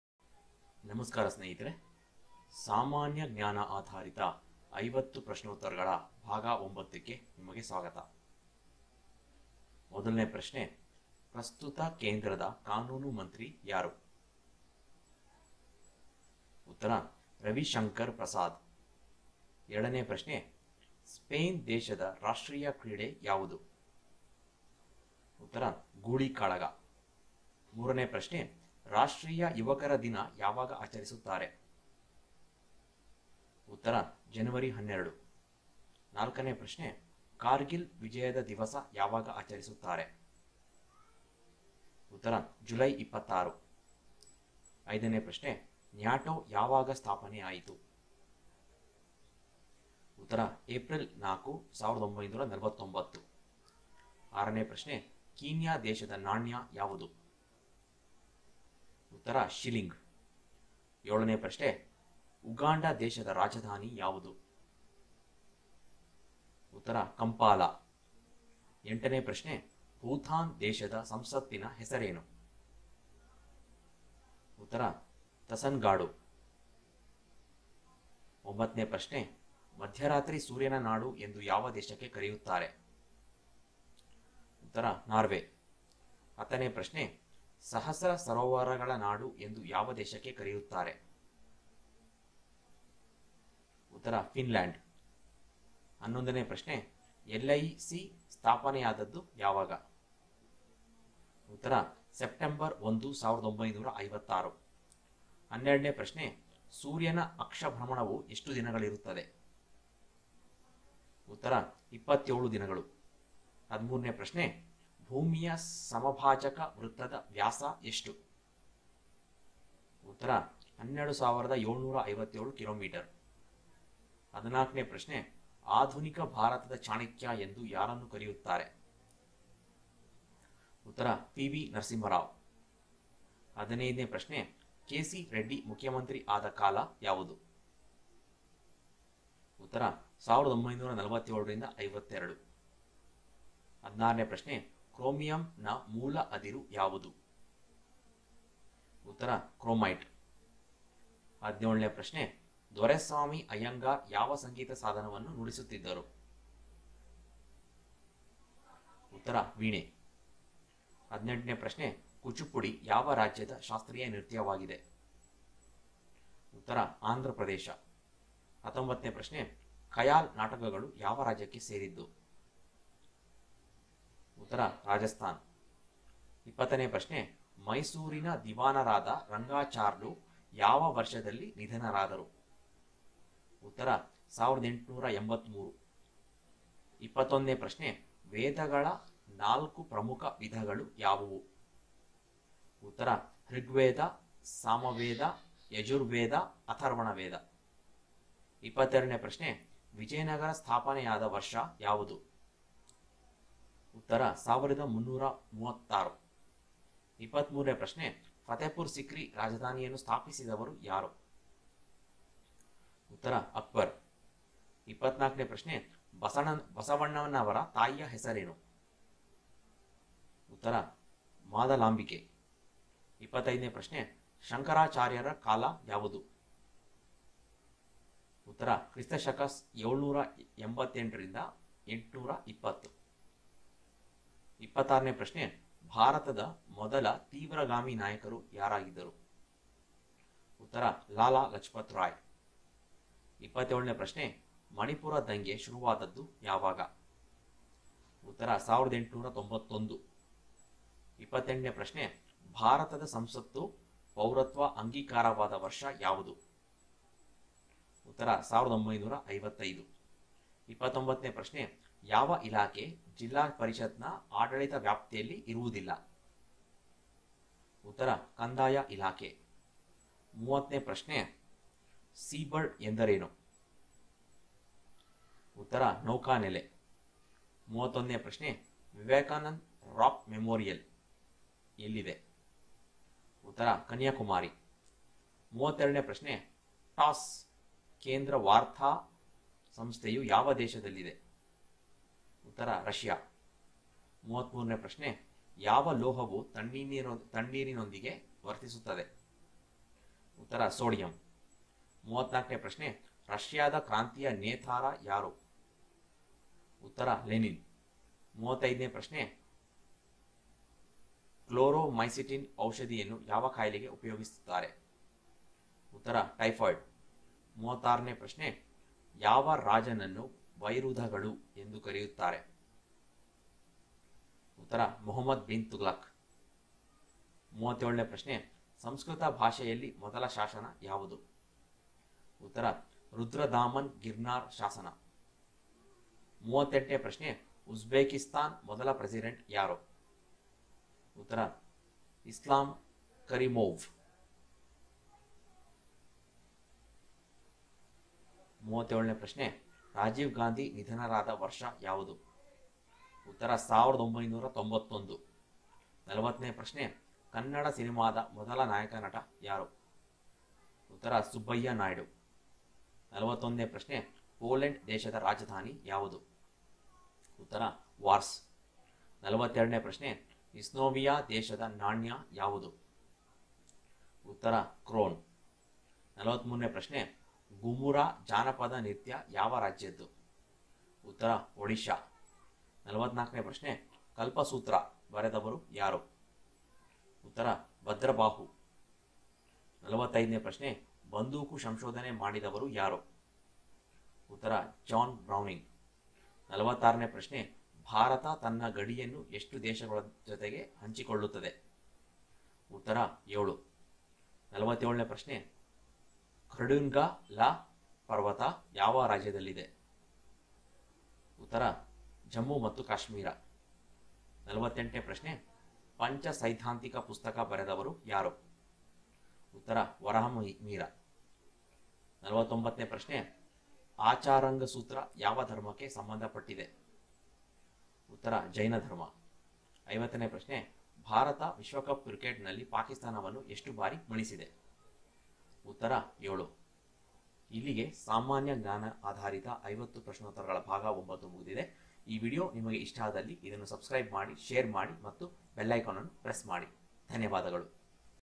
ಸಾಮಾನ್ಯ ಜ್ಞಾನ ಆಧಾರಿತ ೫೦ ಪ್ರಶ್ನೋತ್ತರಗಳು (General Knowledge important 50 Questions) kannada-part 9
kpsc ibps competitive exams#infoshop
In this vedio we have provided 50 important questions(part9) with answers for General knowledge(GK). This is useful for all kinds of government exams